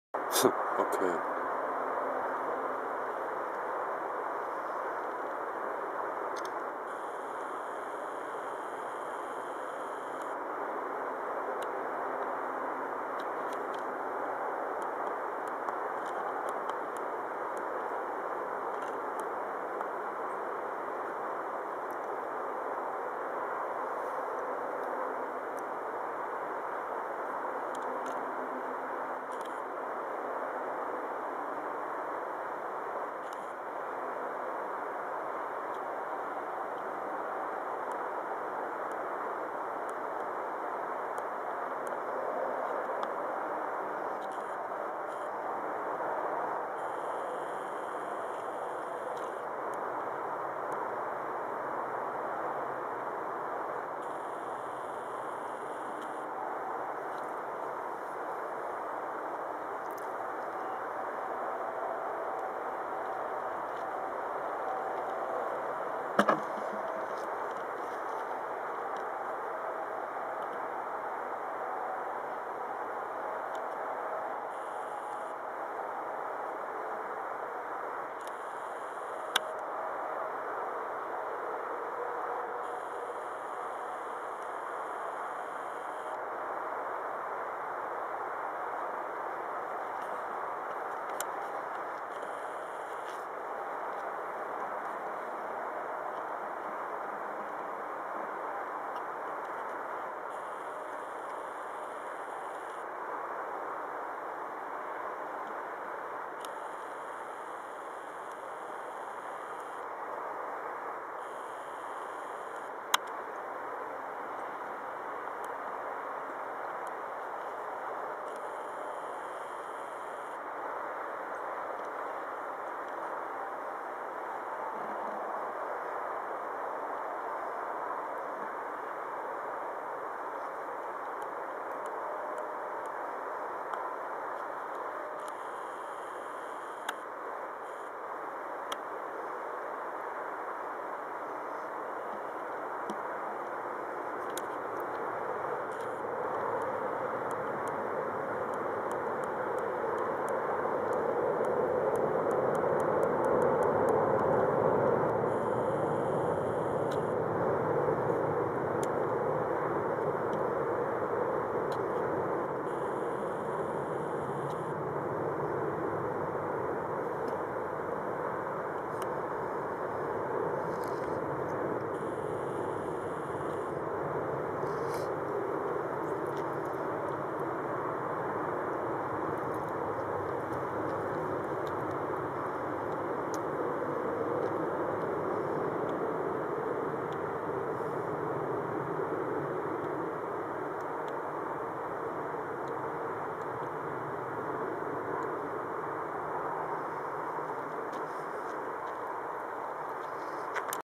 nächtliche Chemtrails
überall blitzen Flugzeuge und ein Blick auf den Mond zeigt sehr deutlich wie systematisch rechtwinklig gesprüht wird!